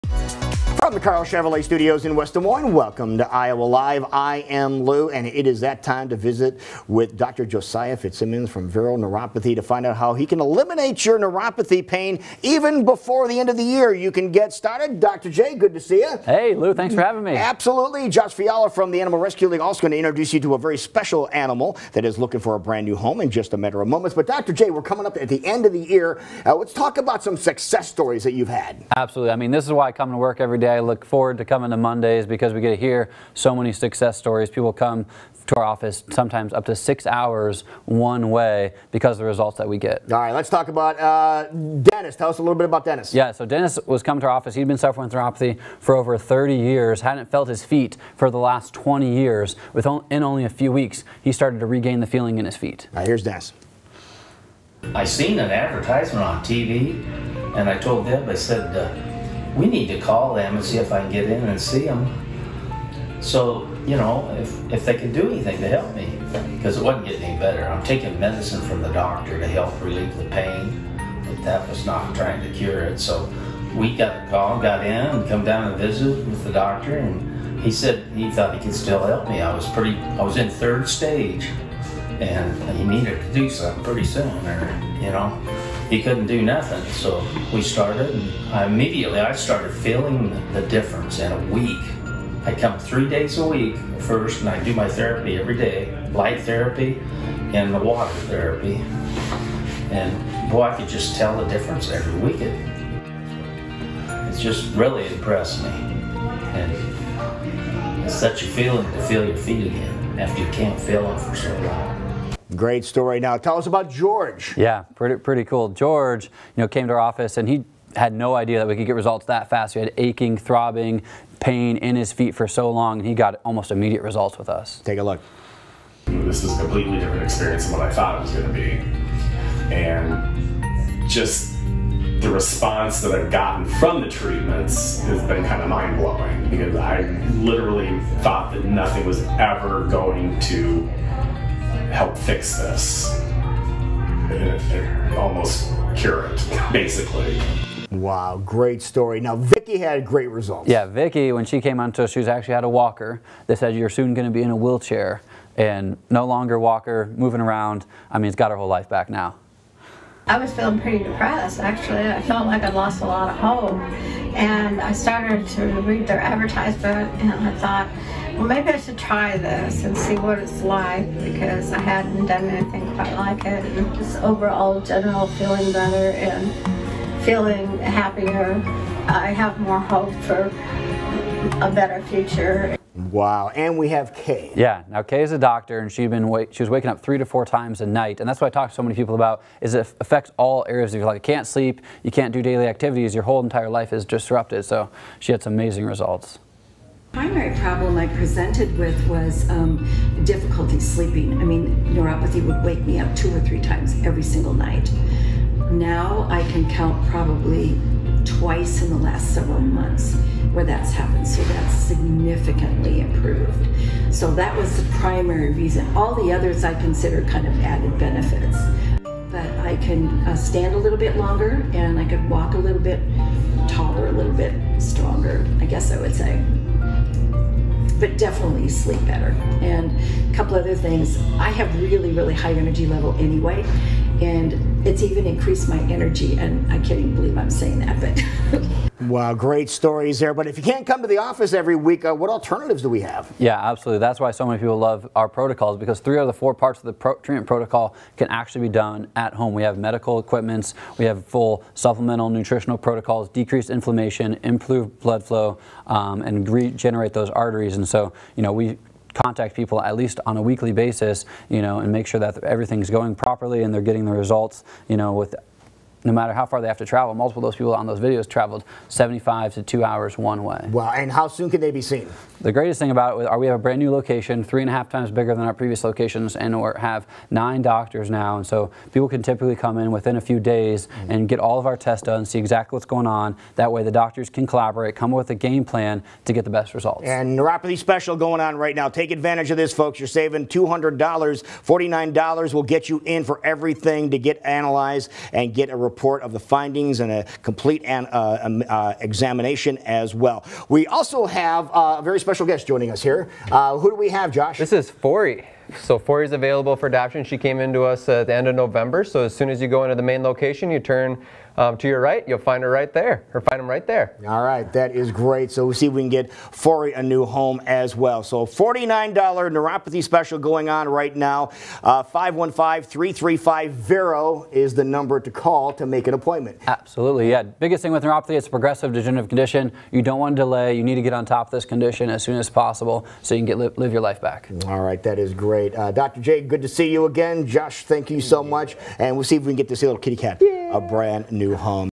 Vero Neuropathy and Their Patients Pain Free Success Stories | Paid Content
Dr. Josiah Fitzsimmons shares his patients success stories and possible treatment plan options | Paid Content